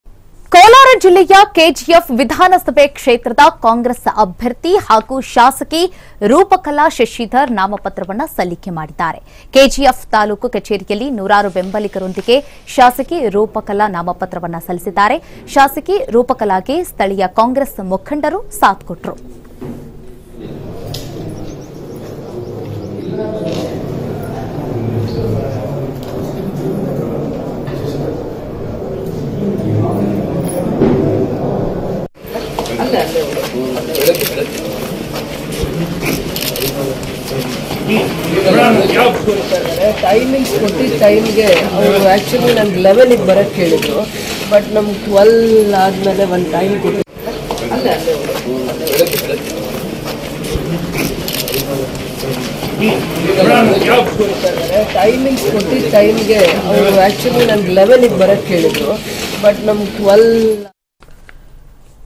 ಕೆಜಿಎಫ್ ವಿಧಾನಸಭಾ ಕ್ಷೇತ್ರದ ಕಾಂಗ್ರೆಸ್ ಅಭ್ಯರ್ಥಿ ಹಾಗೂ ಶಾಸಕಿ ರೂಪಕಲಾ ಶಶಿಧರ್ ನಾಮಪತ್ರ ಸಲ್ಲಿಕೆ ಮಾಡಿದ್ದಾರೆ!
Btv News Kannada : ಕೋಲಾರ ಜಿಲ್ಲೆಯ ಕೆಜಿಎಫ್ ವಿಧಾನಸಭಾ ಕ್ಷೇತ್ರದ ಕಾಂಗ್ರೆಸ್ ಅಭ್ಯರ್ಥಿ ಹಾಗೂ ಶಾಸಕಿ ರೂಪಕಲಾ ಶಶಿಧರ್ ನಾಮಪತ್ರ ಸಲ್ಲಿಕೆ ಮಾಡಿದ್ದಾರೆ!